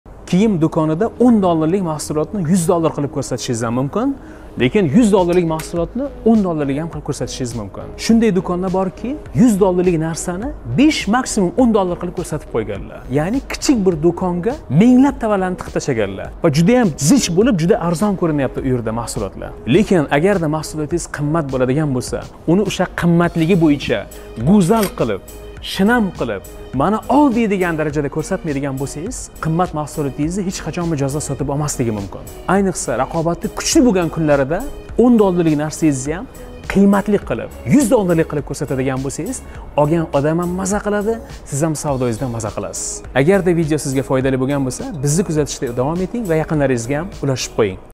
Do'koningizni necha pullik qilib ko'rsatyapsiz?
Kiyim savdosida narx va taqdimot o'rtasida qanday umumiy bog'liqlik mavjud?

‼️ Biznesdagi eng foydali savdo qanday yo'lga qo'yiladi?

✅ 10$ lik mahsulotni 100$ga sotish sirlari.

Bugun sizlar bilan xuddi shu xususida gaplashamiz!

💫 Murat Azamov — 18 yillik xalqaro tadbirkor va biznes motivator!

O'z savollaringizni izoh ostida qoldirishingiz mumkin